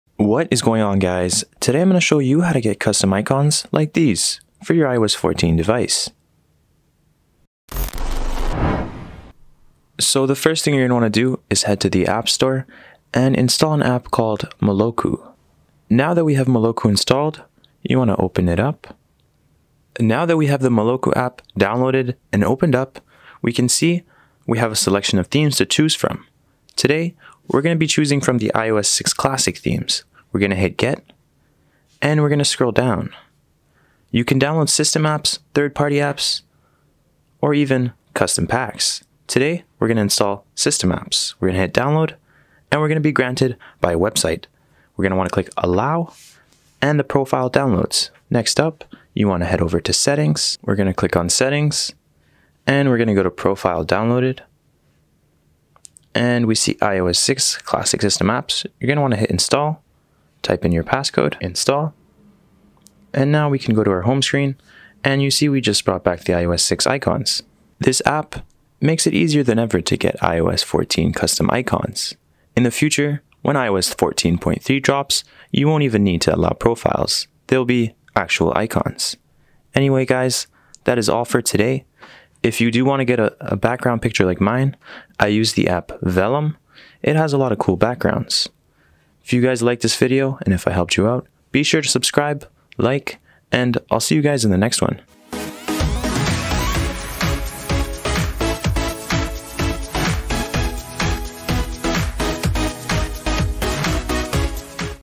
Get Custom Icon Themes on Your iPhone - No Shortcuts (IOS 14.3)
When IOS 14.3 Comes out, you will no longer be redirected through the shortcuts app. This tutorial explains how to get custom Themes on your IOS 14 Device. For now, this is probably one of the best theme apps for your iPhone. Let me know if you guys have any questions I'll get back to you in the comments!